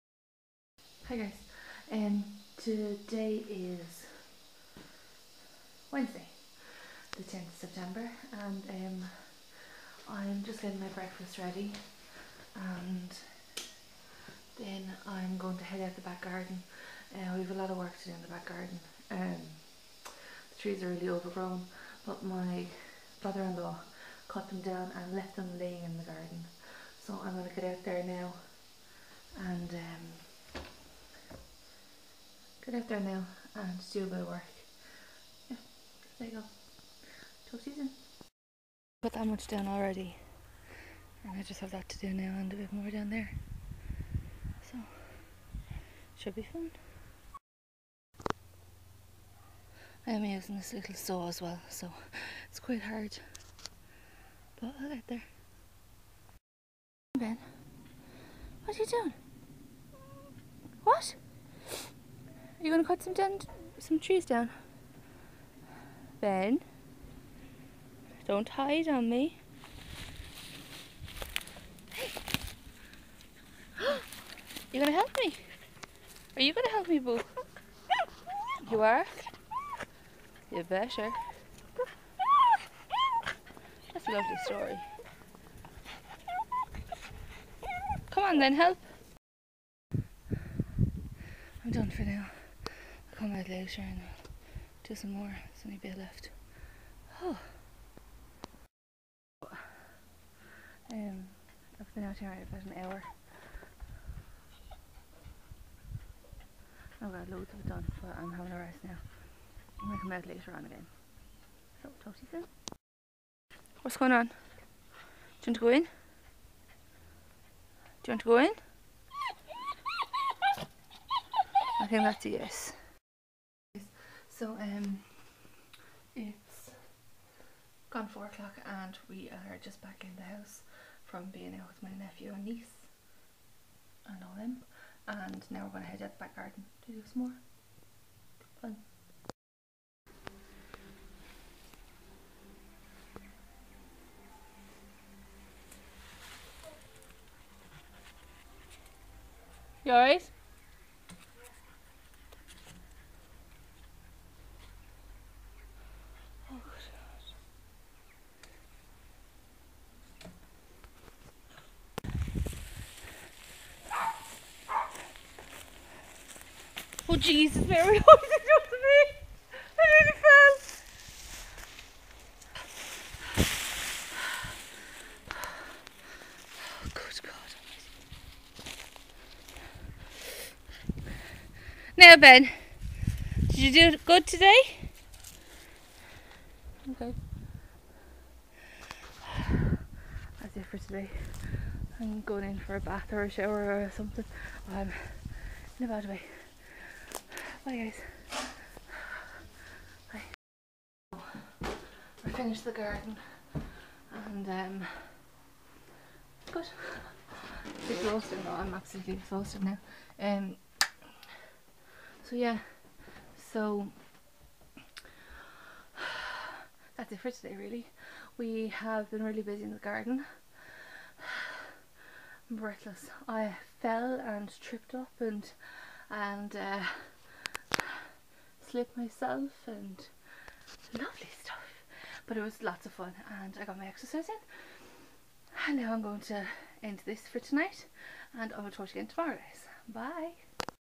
Cutting down trees ¦Vlogtember Day 10¦ 10 09 2014
I spent the day in our back garden cutting down trees and fixing up the garden a little bit. It was hard work but rewarding at the same time.